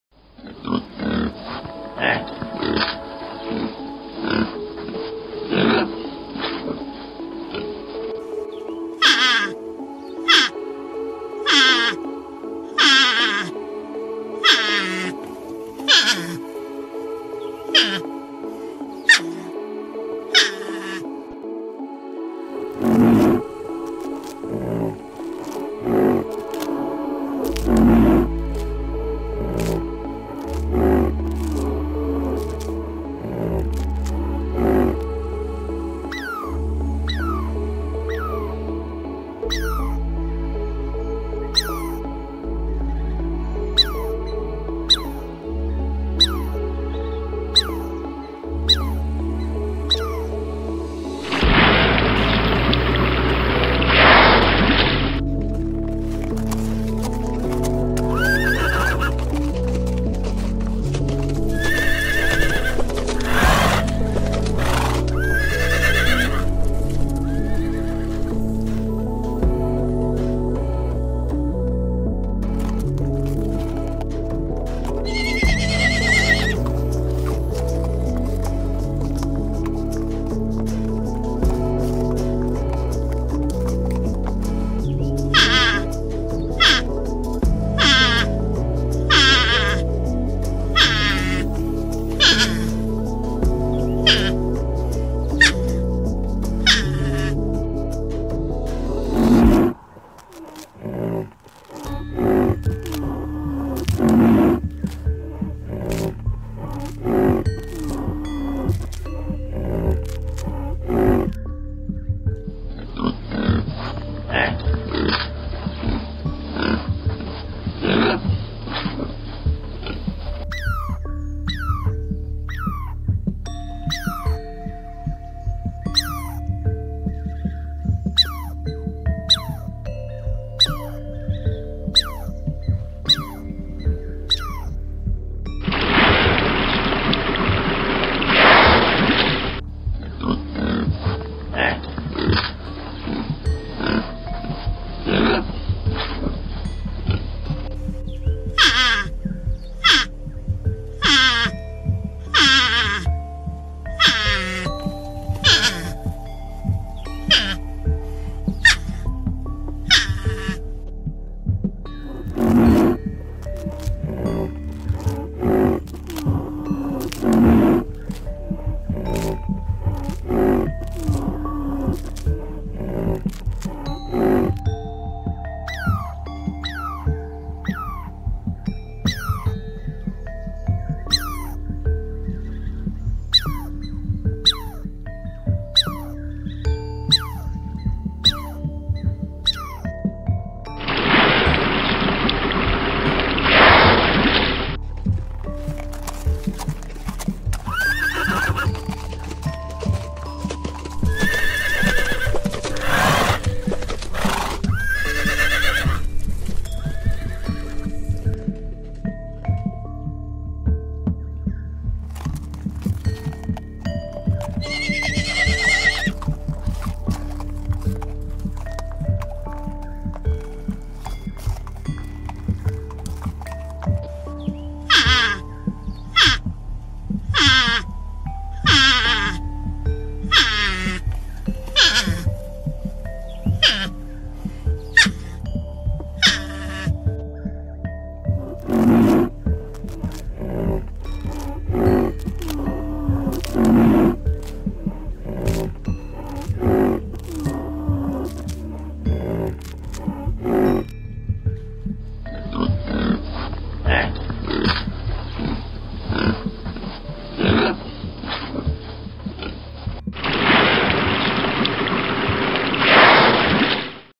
Rhino Grunts and Squirrel Barks: A Compilation of Animal Sounds
In this video, we explore the sounds of some of the world's most fascinating animals. First up, we have the zebra, known for its distinctive black and white stripes. Have you ever wondered how a zebra sounds like? Well, wonder no more! We have captured zebra sounds free effects for you to listen to. From the gentle whinnying of a mare to the loud and piercing barks of a stallion, the zebra sounds loud and varied.

Next, we move on to the grey squirrel. These small but mighty animals are known for their quick movements and playful behavior. But did you know that they also make a variety of noises? Listen closely and you can hear them barking, chattering, and even screaming. We have captured squirrel barking noises for you to enjoy.

If you're looking for a more soothing animal sound, then the rhino may not be for you. Rhinos are wild animals that can be heard from miles away with their loud and deep grunts. In this video, you'll hear rhino sounds effects that make you feel like you're right there in the African savanna. From the gentle snorts of a mother to the aggressive grunts of a male, the rhinoceros sound effect is both impressive and intimidating.

So sit back, relax, and enjoy the real zebra noises real, grey squirrel sounds, and rhino sound effects in this amazing video.

Video Length: 04:25

⏰ SCHEDULE:
No Schedule